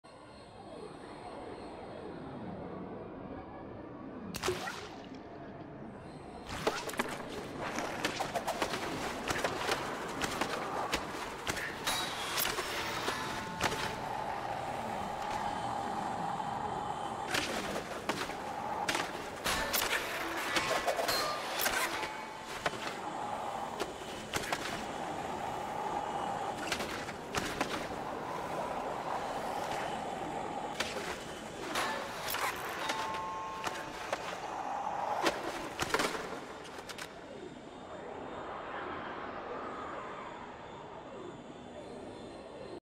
252,876 High Score Replay Gold I - Season 16 - Event 25
This is my highscore replay for Gold I, season 16, event 25 in the Gnarvana League.